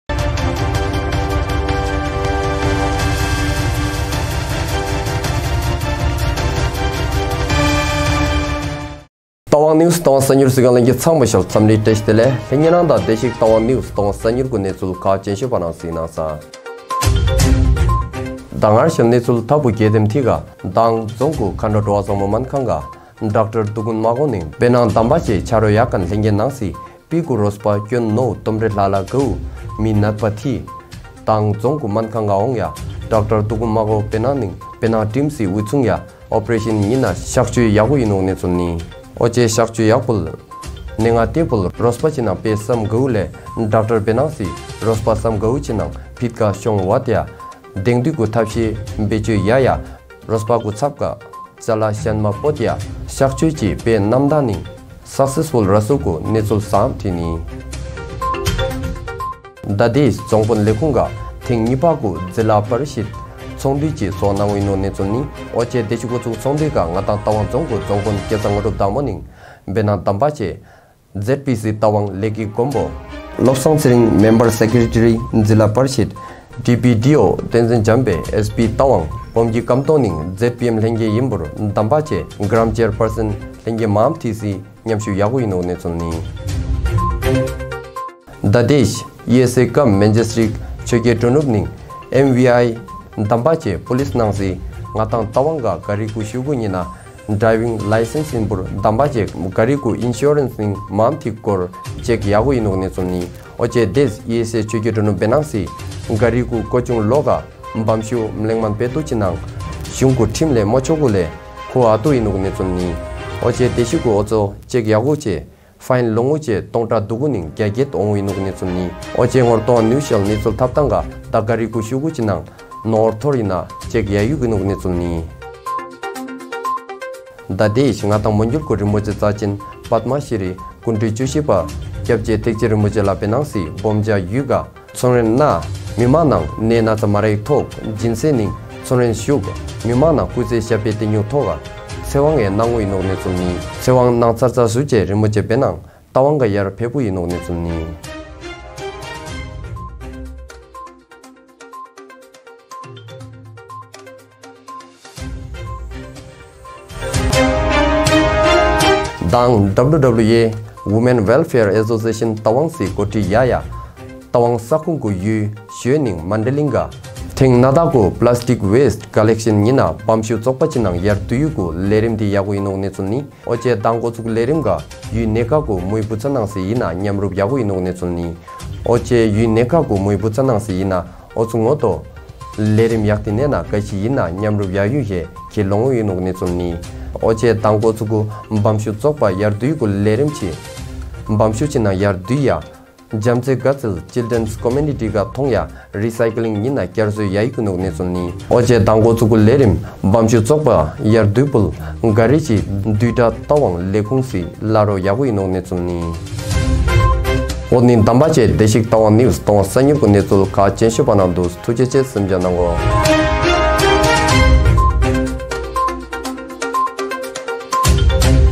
Date 13 May 2022 (Fri) Daily News Bulletin ( in monpa )
NEWS HEADLINES ( in monpa )Second meeting to review progress of developmental activities of 14th & 15th Finance Commission under Tawang Zila Parishad was held MV checking was conducted by District Administration # A total hip replacement was performed @ KDS.